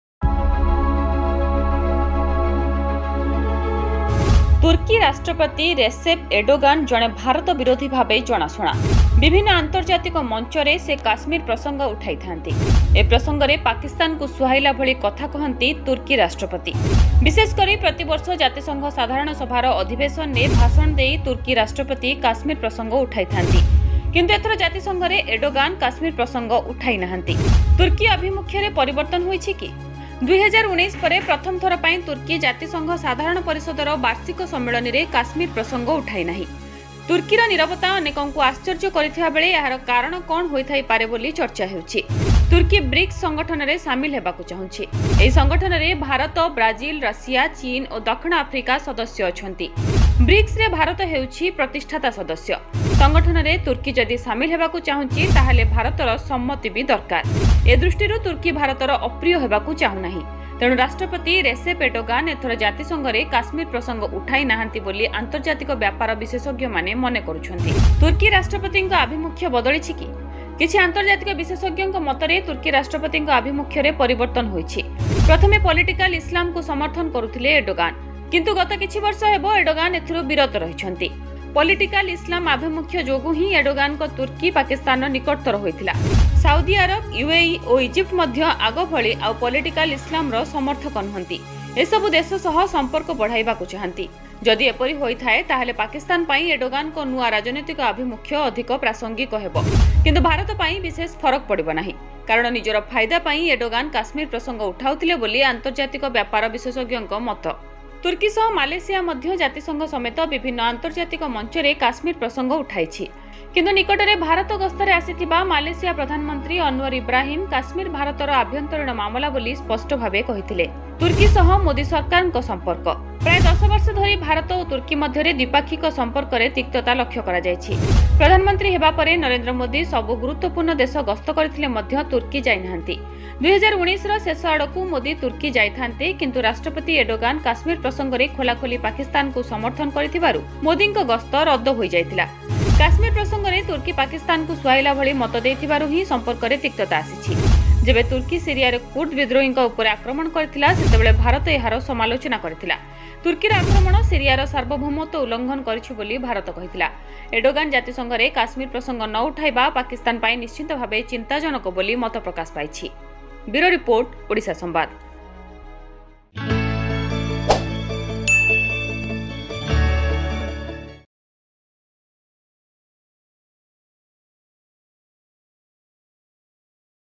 Why Did Turkish President Omit Kashmir Mention At UNGA For First Time Since 2019?
ତୁର୍କୀ ରାଷ୍ଟ୍ରପତି ରେସେପ ଏର୍ଡୋଗାନ ଜଣେ ଭାରତ ବିରୋଧୀ ଭାବେ ଜଣାଶୁଣା । ବିଭିନ୍ନ ଆନ୍ତର୍ଜାତିକ ମଂଚରେ ସେ କାଶ୍ମୀର ପ୍ରସଙ୍ଗ ଉଠାଇ ଥାଆନ୍ତି